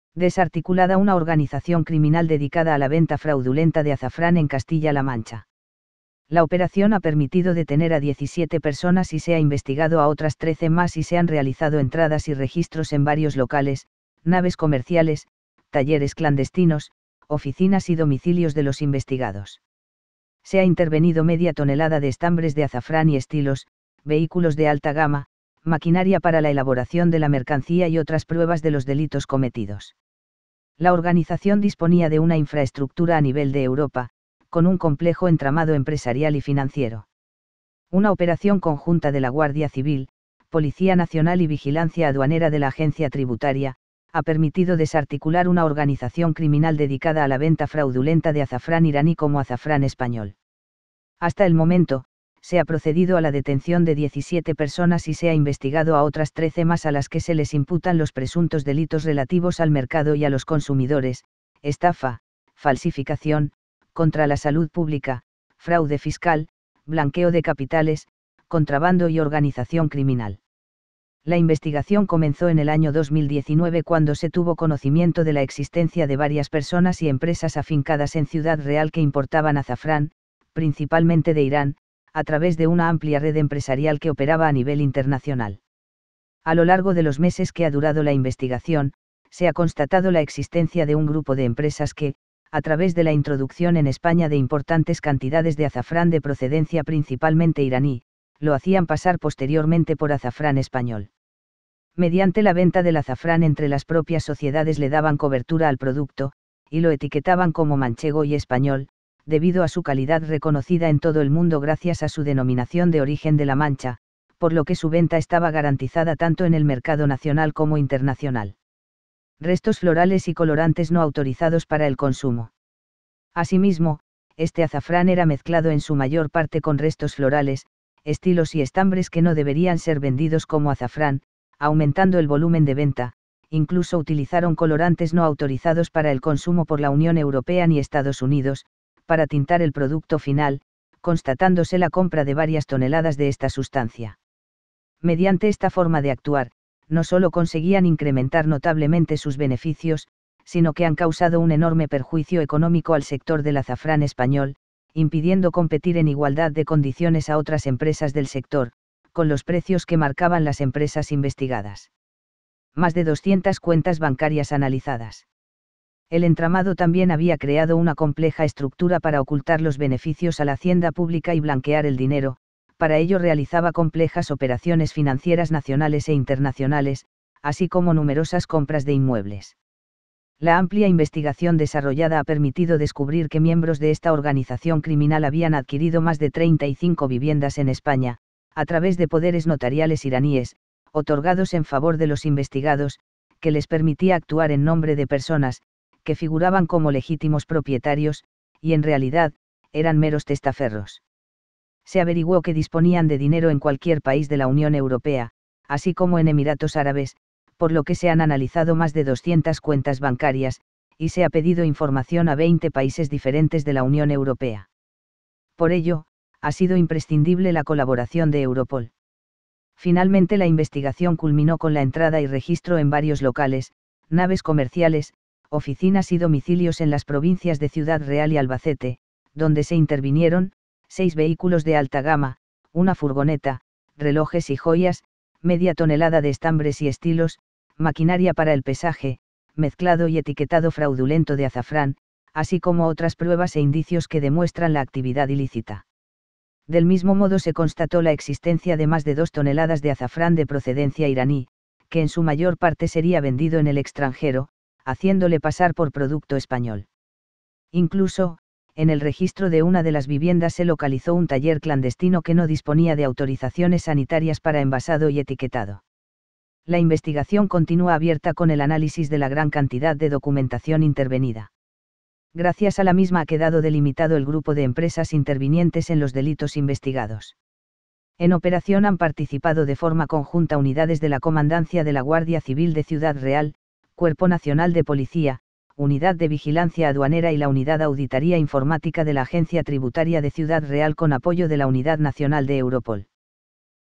💥Echaban COLORANTE al AZAFRÁN iraní para venderlo como español💥 🛑17 detenidos en Ciudad Real🛑💥🟥🟨🟥
Desarticulada una organización criminal dedicada a la venta fraudulenta de azafrán en Castilla la Mancha

La operación ha permitido detener a 17 personas y se ha investigado a otras 13 más y se han realizado entradas y registros en varios locales, naves comerciales, talleres clandestinos, oficinas y domicilios de los investigados

Se ha intervenido media tonelada de estambres de azafrán y estilos, vehículos de alta gama, maquinaria para la elaboración de la mercancía y otras pruebas de los delitos cometidos

La organización disponía de una infraestructura a nivel de Europa, con un complejo entramado empresarial y financiero

Una operación conjunta de la Guardia Civil, Policía Nacional y Vigilancia Aduanera de la Agencia Tributaria, ha permitido desarticular una organización criminal dedicada a la venta fraudulenta de azafrán iraní como azafrán español. Hasta el momento, se ha procedido a la detención de 17 personas y se ha investigado a otras 13 más a las que se les imputan los presuntos delitos relativos al mercado y a los consumidores, estafa, falsificación, contra la salud pública, fraude fiscal, blanqueo de capitales, contrabando y organización criminal.

La investigación comenzó en el año 2019 cuando se tuvo conocimiento de la existencia de varias personas y empresas afincadas en Ciudad Real que importaban azafrán, principalmente de Irán, a través de una amplia red empresarial que operaba a nivel internacional. A lo largo de los meses que ha durado la investigación, se ha constatado la existencia de un grupo de empresas que, a través de la introducción en España de importantes cantidades de azafrán de procedencia principalmente iraní, lo hacían pasar posteriormente por azafrán español. Mediante la venta del azafrán entre las propias sociedades le daban cobertura al producto, y lo etiquetaban como manchego y español, debido a su calidad reconocida en todo el mundo gracias a su denominación de origen de La Mancha, por lo que su venta estaba garantizada tanto en el mercado nacional como internacional.

Restos florales y colorantes no autorizados para el consumo

Asimismo, este azafrán era mezclado en su mayor parte con restos florales, estilos y estambres que no deberían ser vendidos como azafrán, aumentando el volumen de venta; incluso utilizaron colorantes no autorizados para el consumo por la Unión Europea ni Estados Unidos, para tintar el producto final, constatándose la compra de varias toneladas de esta sustancia.

Mediante esta forma de actuar, no solo conseguían incrementar notablemente sus beneficios, sino que han causado un enorme perjuicio económico al sector del azafrán español, impidiendo competir en igualdad de condiciones a otras empresas del sector, con los precios que marcaban las empresas investigadas.

Más de 200 cuentas bancarias analizadas

El entramado también había creado una compleja estructura para ocultar los beneficios a la Hacienda Pública y blanquear el dinero, para ello realizaba complejas operaciones financieras nacionales e internacionales, así como  numerosas compras de inmuebles. La amplia investigación desarrollada ha permitido descubrir que miembros de esta organización criminal habían adquirido más de 35 viviendas en España, a través de poderes notariales iraníes, otorgados en favor de los investigados, que les permitía actuar en nombre de personas, que figuraban como legítimos propietarios, y en realidad, eran meros testaferros. Se averiguó que disponían de dinero en cualquier país de la Unión Europea, así como en Emiratos Árabes, por lo que se han analizado más de 200 cuentas bancarias, y se ha pedido información a 20 países diferentes de la Unión Europea. Por ello, ha sido imprescindible la colaboración de EUROPOL.